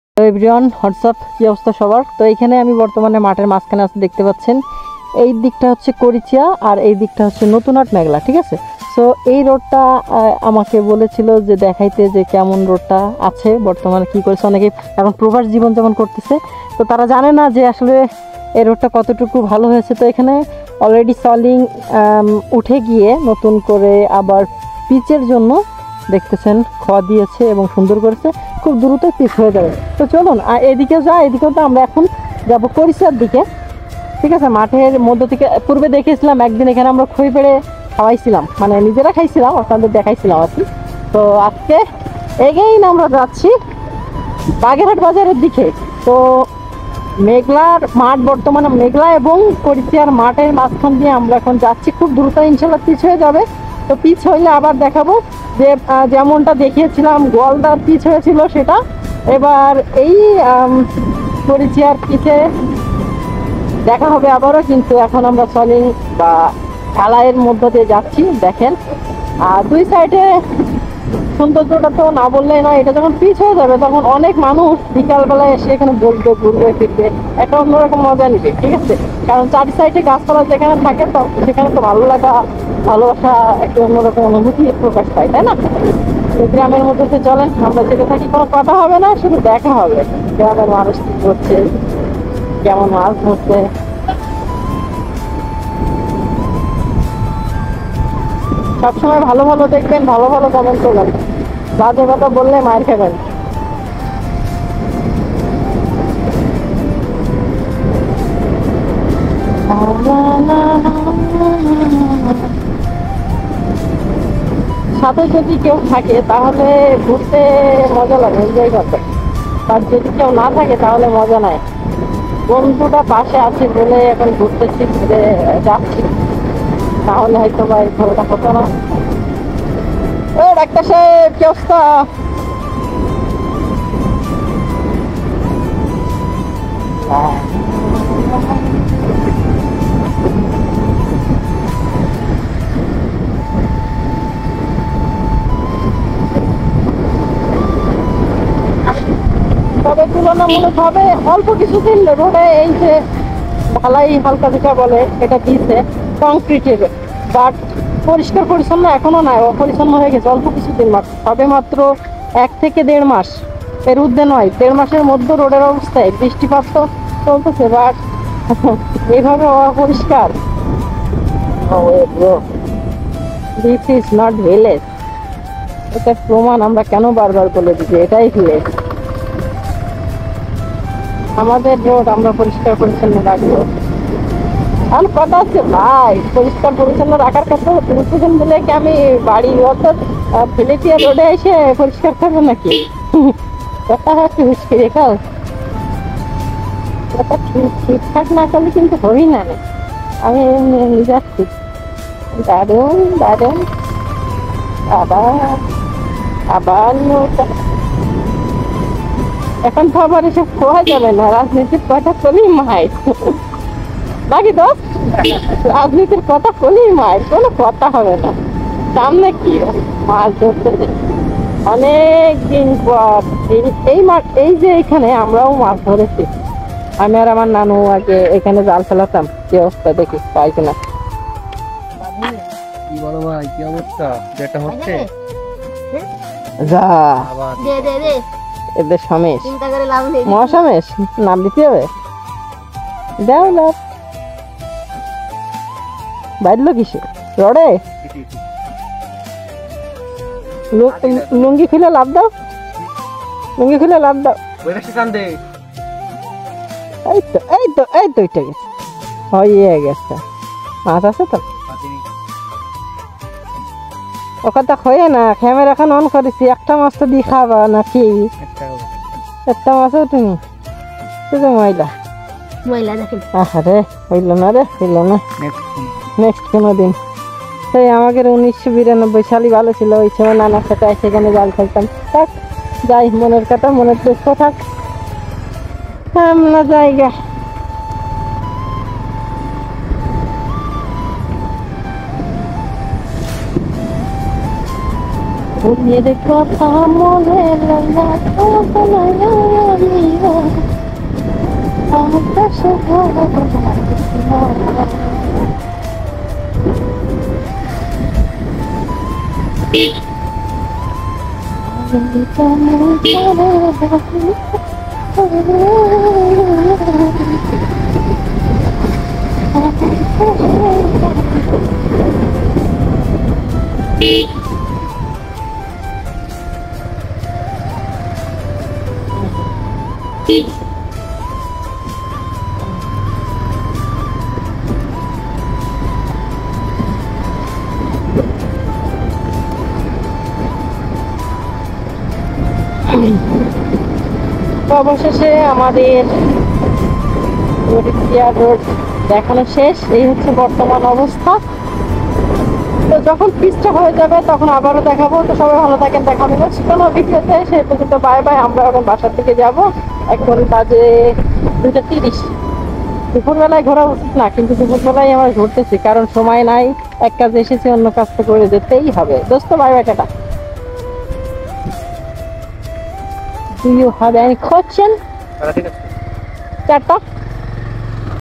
বাগেরহাট বাজার টু করিচিয়া ও করিচিয়া টু মেঘলা এবং নতুনহাট বাজারের গ্রামের রাস্তা কেমন তৈরি হয়েছে..
বাগেরহাট বাজার টু করিচিয়া ও করিচিয়া টু মেঘলা এবং নতুনহাট বাজারের গ্রামের রাস্তা কেমন তৈরি হয়েছে চলুন দেখে আসি!